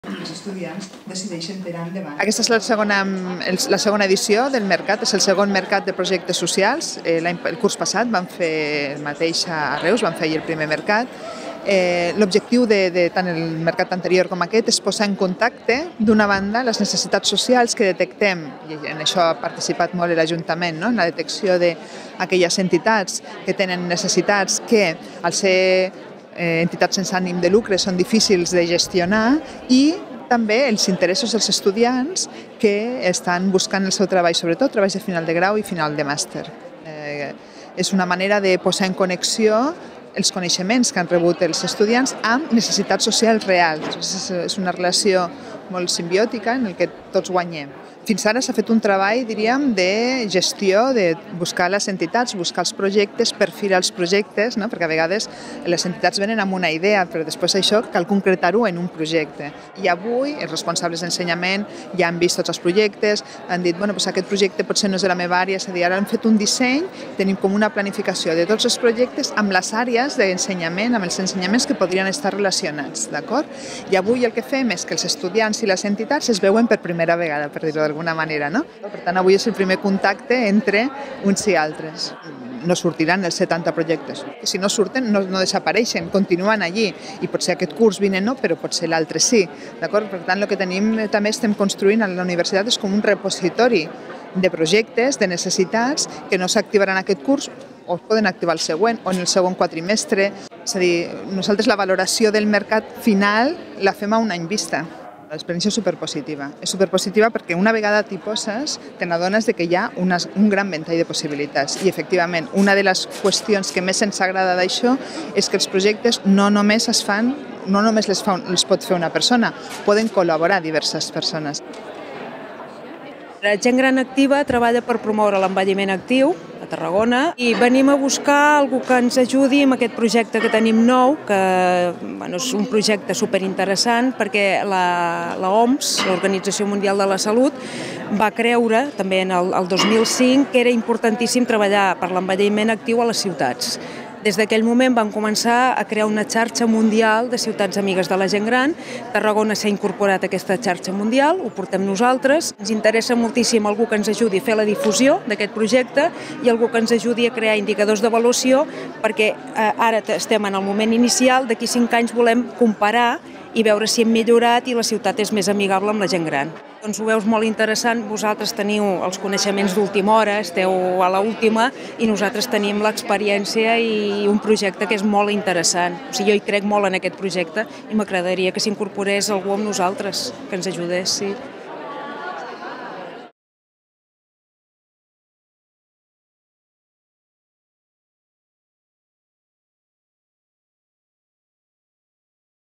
Entitats de Tarragona presenten propostes al II Mercat de Projectes Socials de la URV
La segona edició del Mercat de Projectes Socials s’ha fet al campus Sescelades i ha comptat amb la participació de 23 entitats que han proposat projectes socials, culturals i ambientals que poden ser desenvolupats pels alumnes com a Treballs de Fi de Grau o de Màster. Es tracta d’una activitat del Programa d’Aprenentatge Servei, que enguany s’ha organitzat amb l’Ajuntament de Tarragona i amb el suport del Consell Social de la URV.